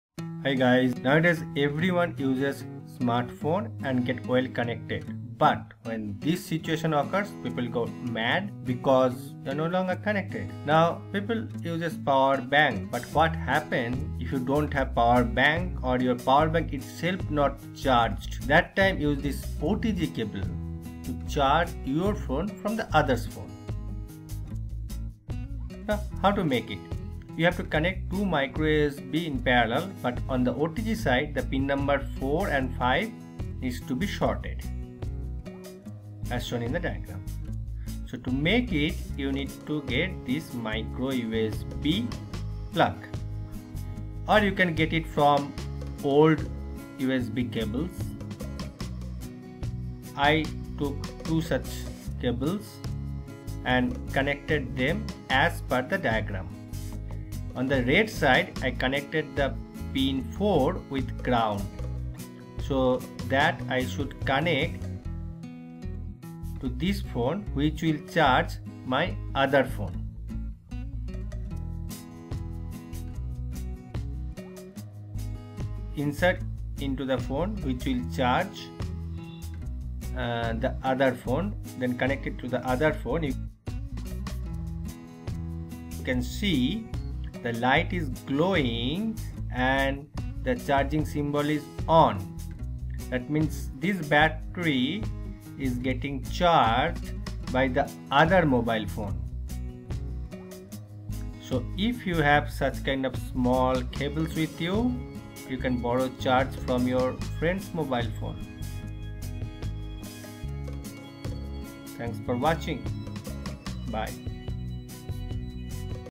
How to make an OTG charging cable to charge the battery of your mobile phone
Here is the alternate way to charge your mobile. This is an OTG charging cable. Very easy to make. You can connect to any OTG phone to get power from it and recharge your mobile phone battery.